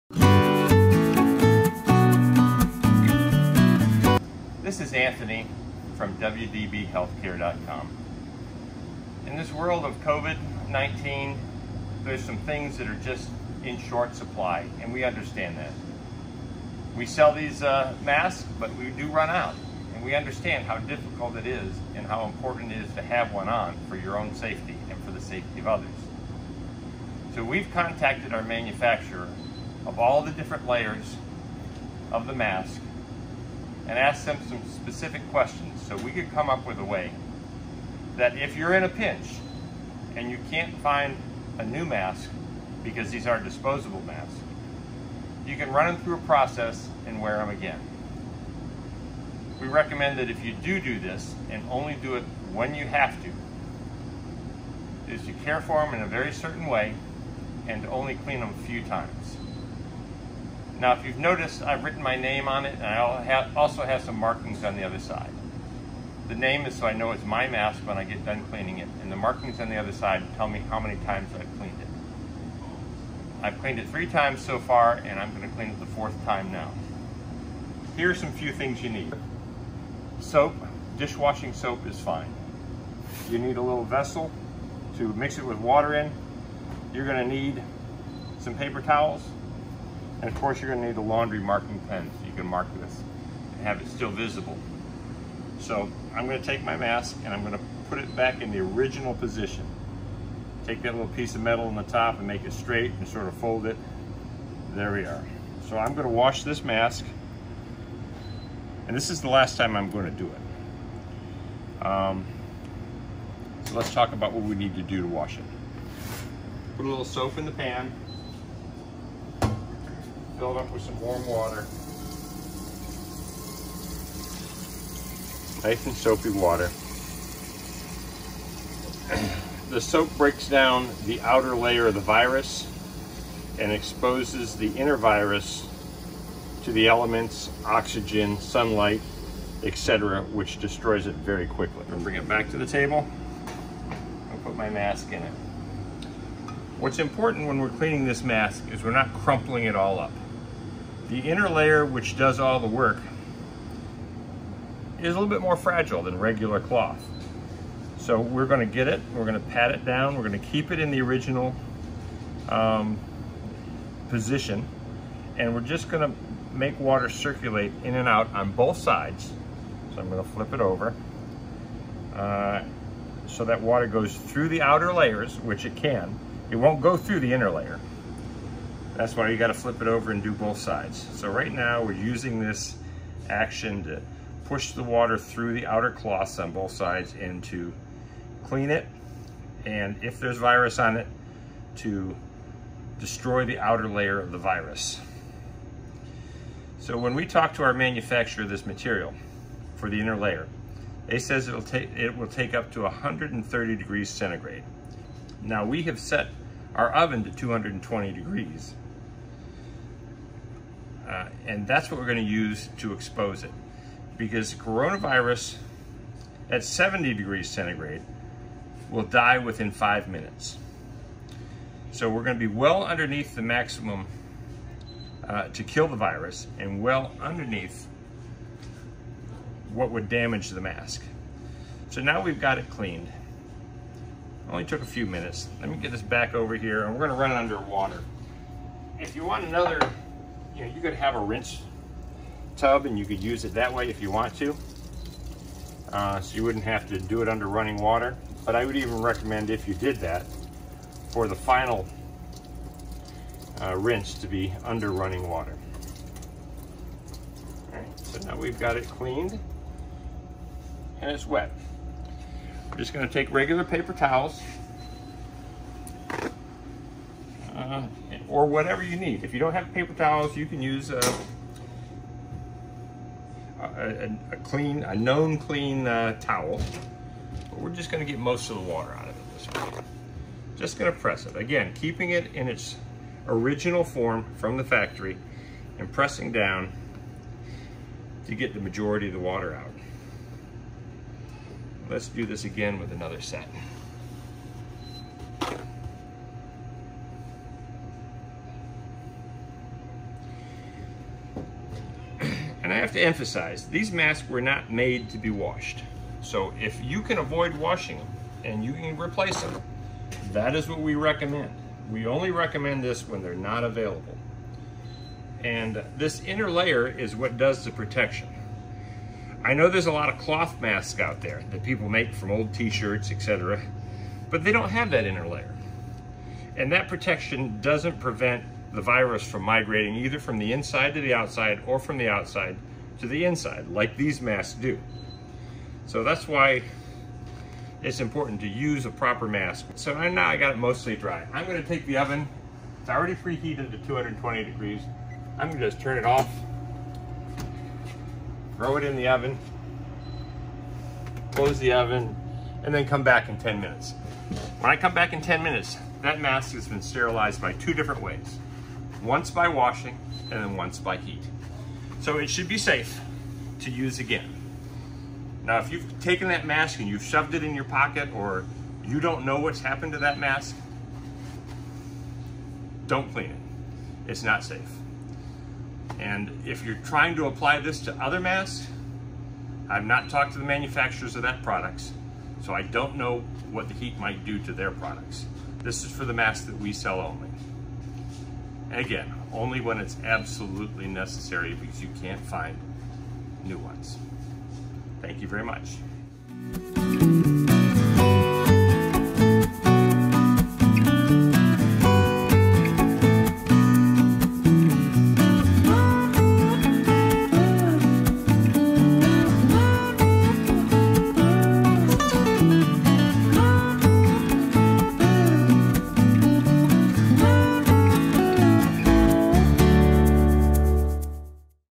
Disposable Mask Washing Video
How to safely clean disposable masks when forced to reuse them.